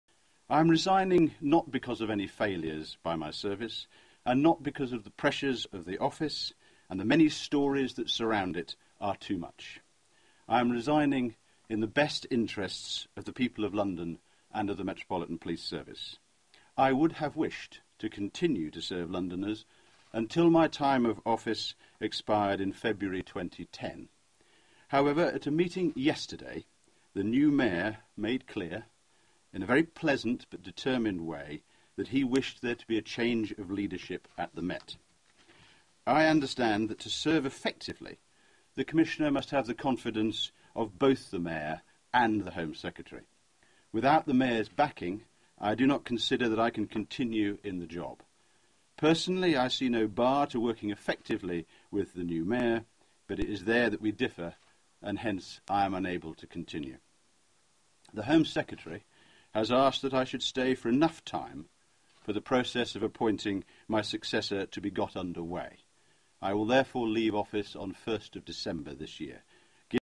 Sir Ian Blair Resigns
Ian Blair Police chief
claims he was pushed out
by funny man Boris Johnson.